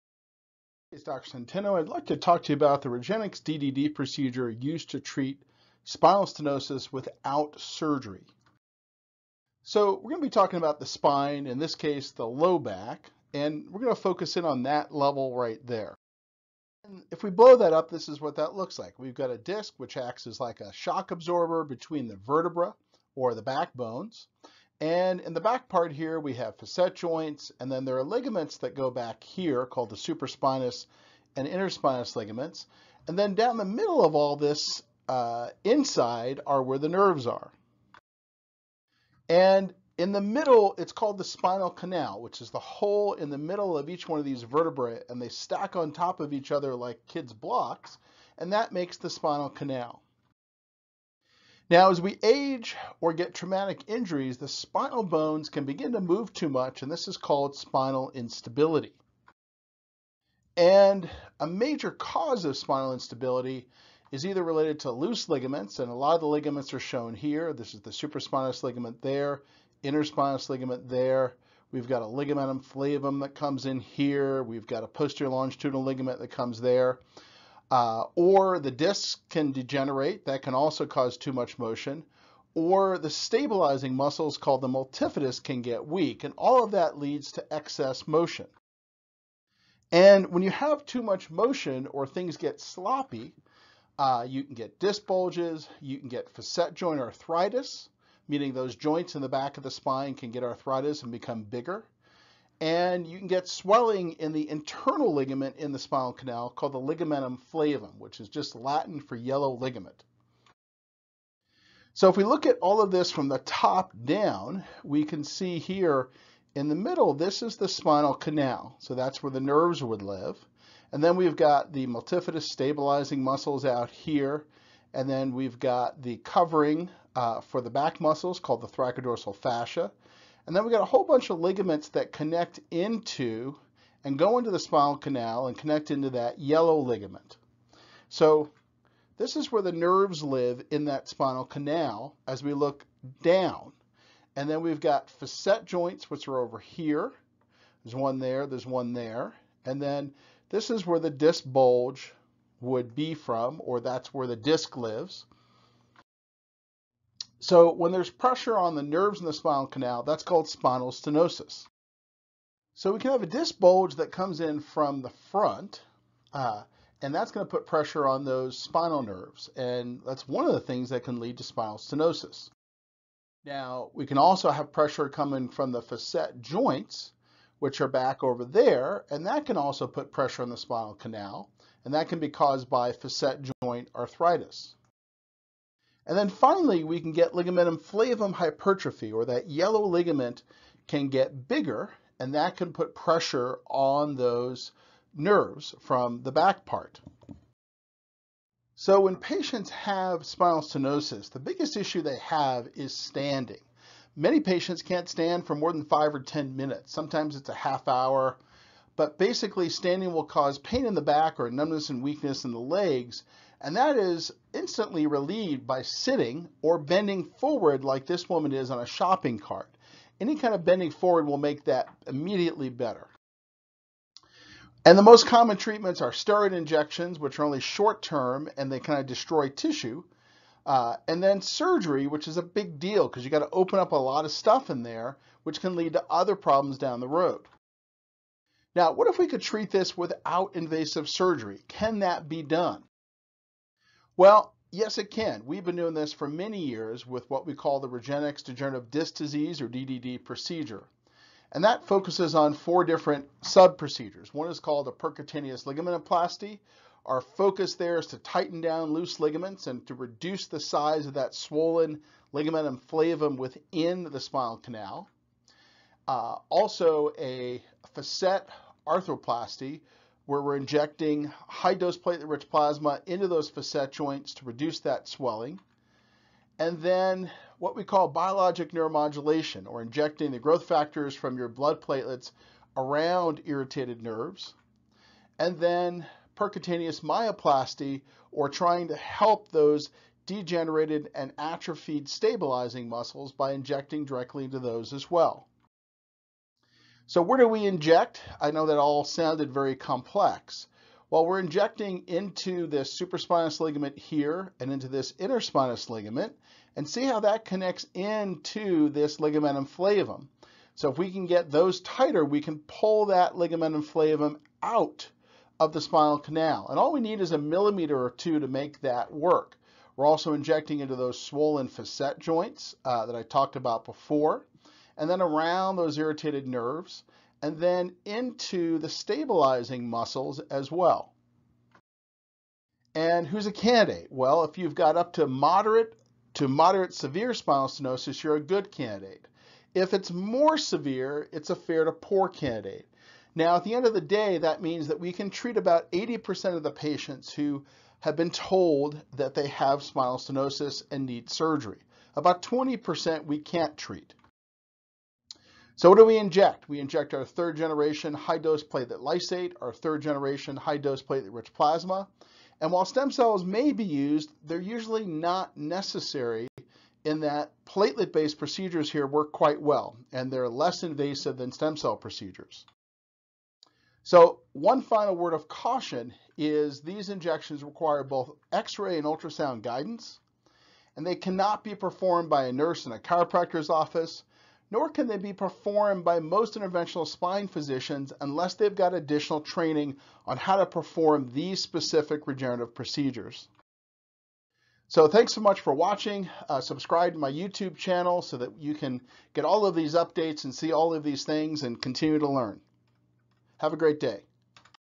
Spinal Stenosis Surgery Alternative Regenexx DDD procedure
How is it possible to avoid spinal stenosis surgery? Dr. Centeno discusses spinal stenosis surgery alternatives that include platelet-rich plasma (PRP), platelet lysate, and stem cells.

If you want to avoid spinal stenosis or degenerative disc disease (DDD), seek a consultation from Centeno-Schultz Clinic or across the US by finding your closest Regenexx provider.